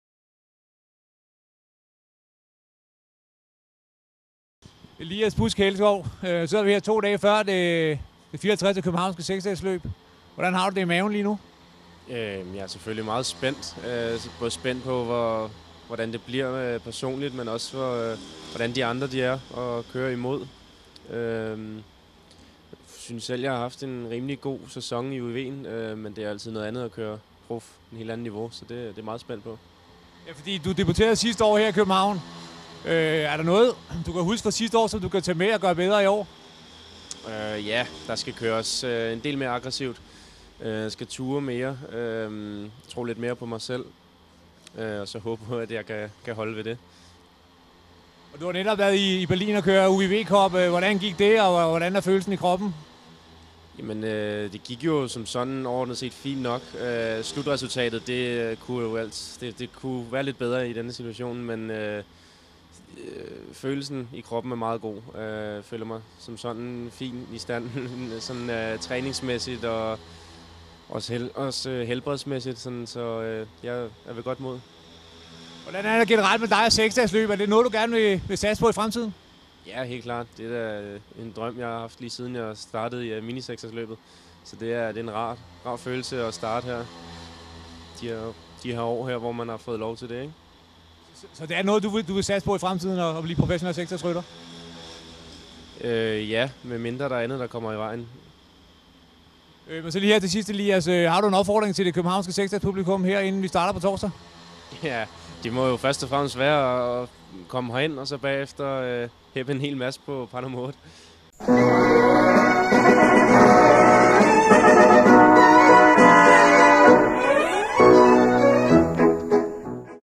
Elias Helleskov Busk
Elias Helleskov Busk 2 dage før det 54. københavnske 6-dagesløb.